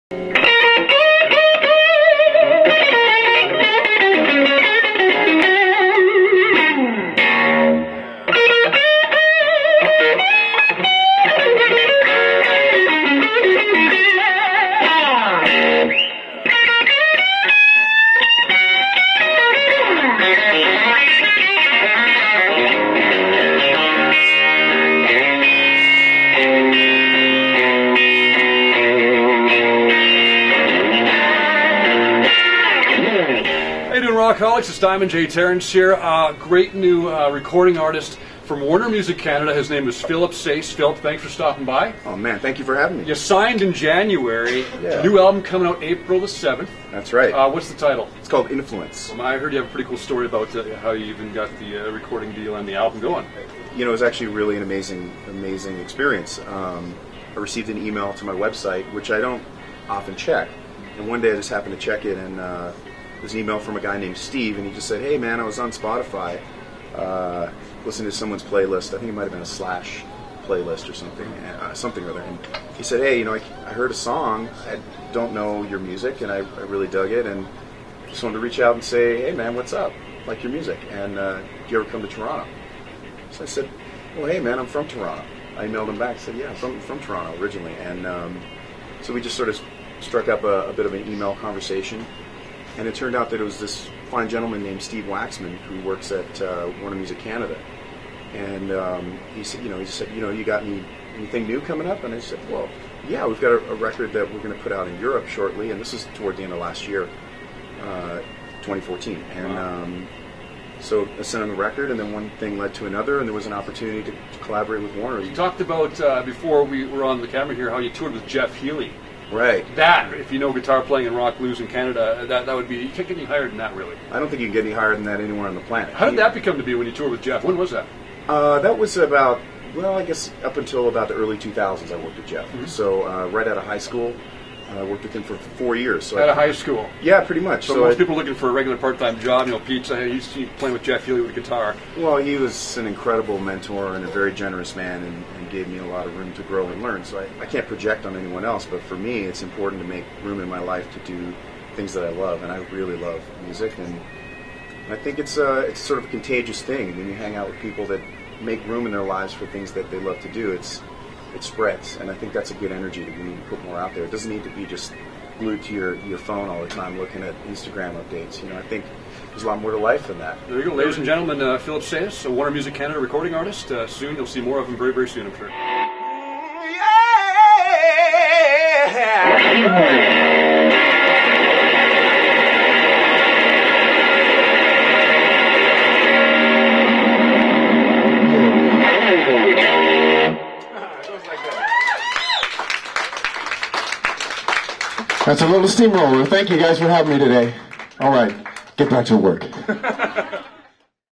Philip Sayce live at CJAY 92
CJAY 92's Diamond J interviews Warner Music Canada's newest artist, Philip Sayce,  in the CJAY 92 studio.